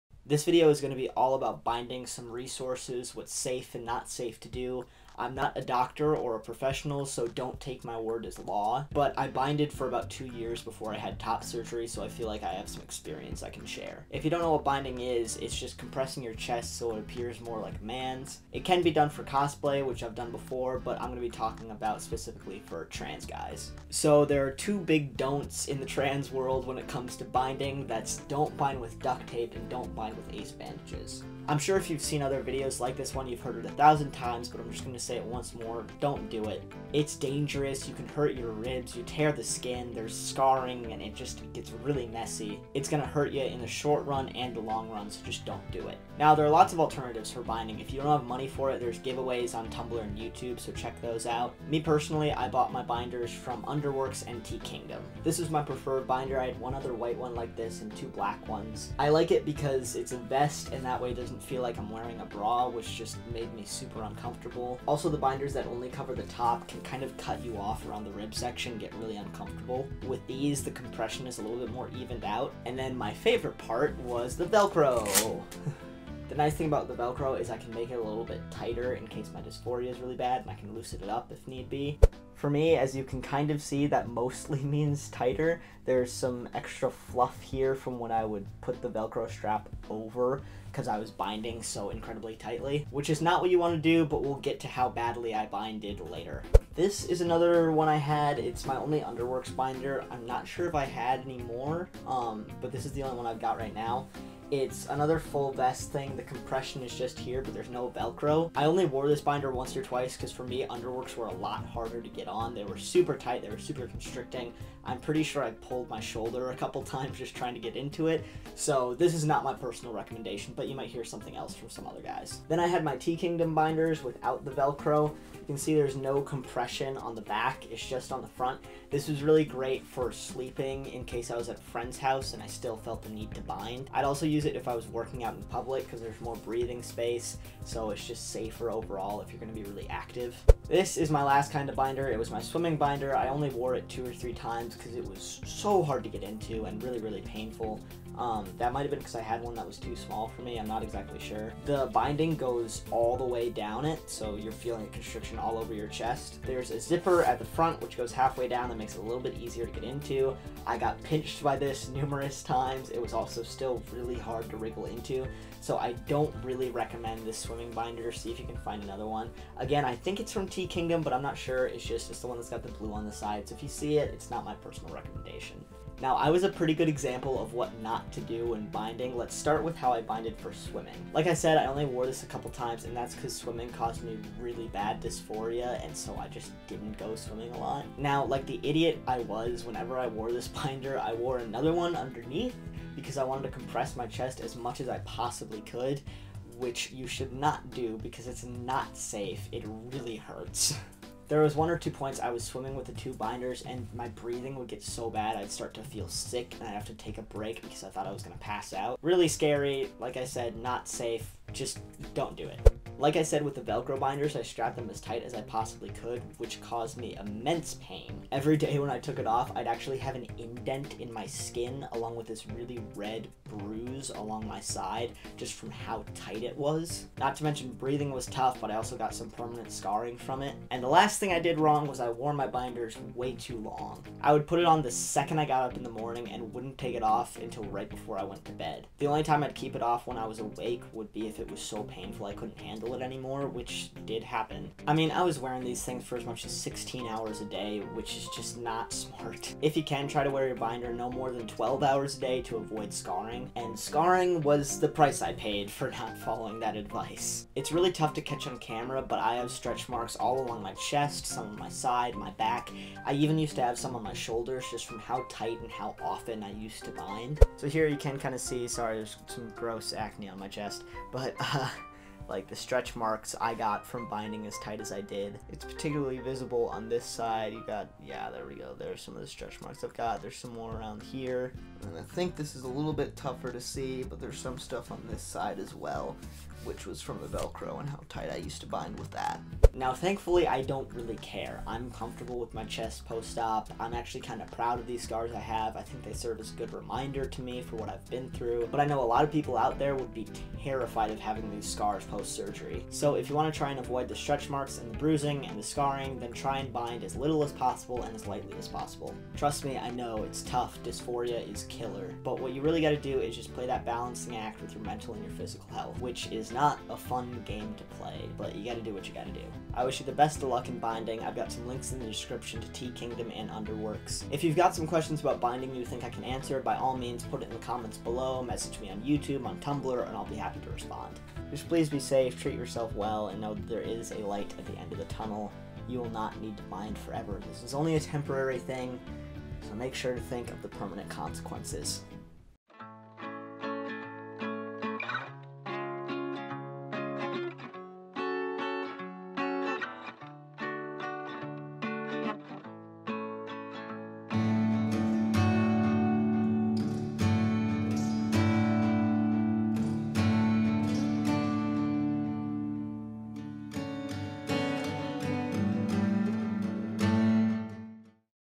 Binding 101 for Transmen
It's so important to be careful when you're doing this kind of stuff. There's a reason I drill it in this video so many times. Beyond the safety factor, I hope my video was informative.